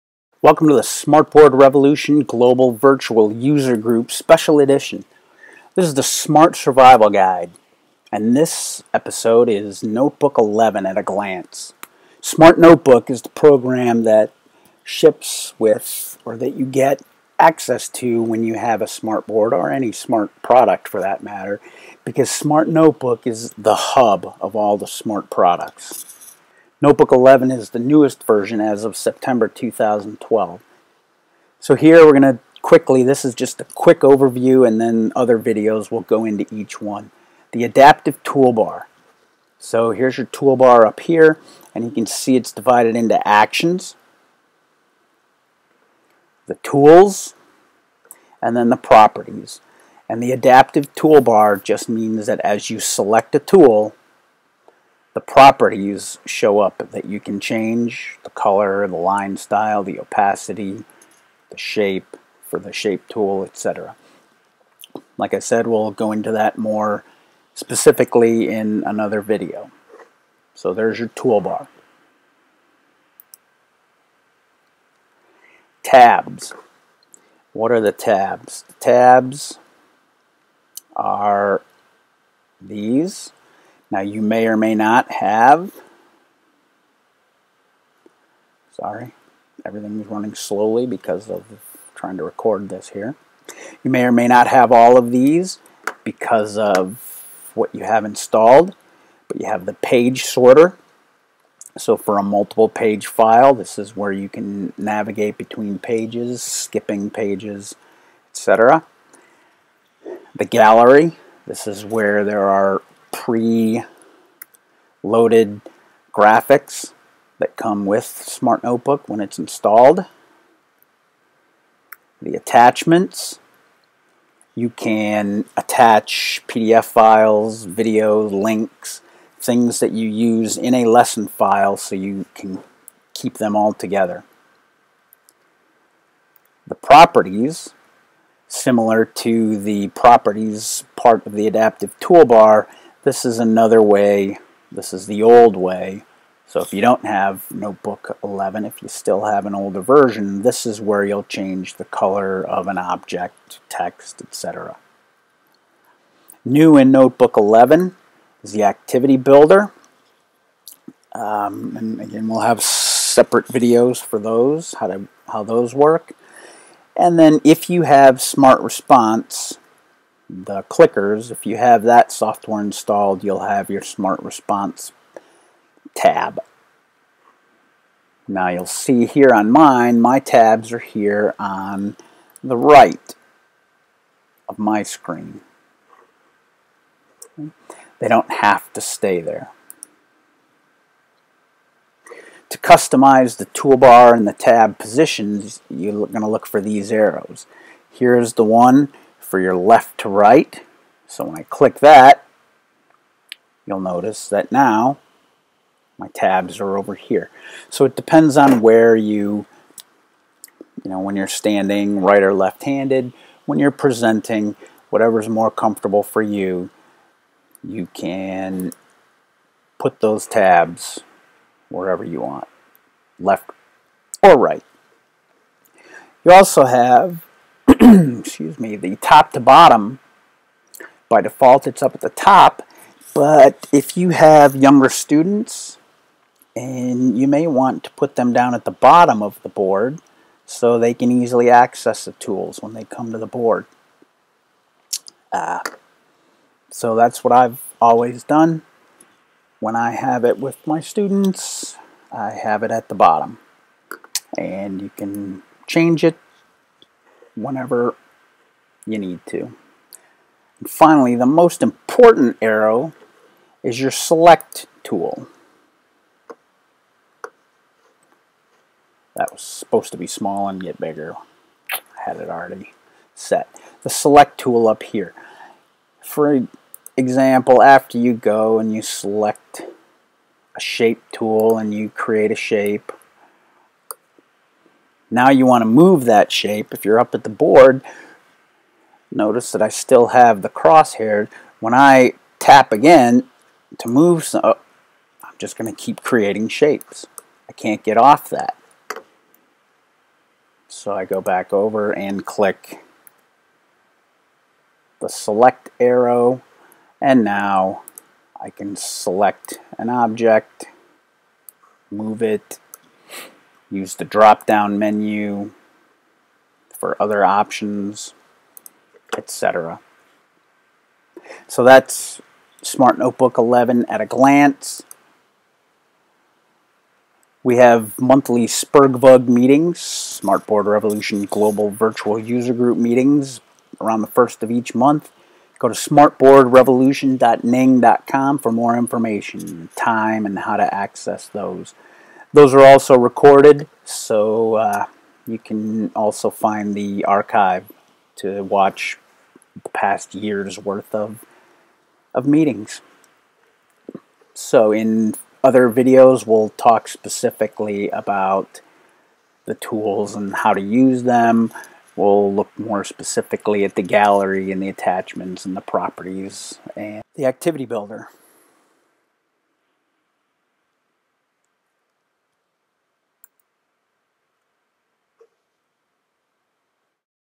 SMART Survival Guide: SMART Notebook 11 at a Glance
Brief overview of SMART Notebook 11 Collaborative Learning Software. Most of the features shown are also part of Notebook 14 and 15.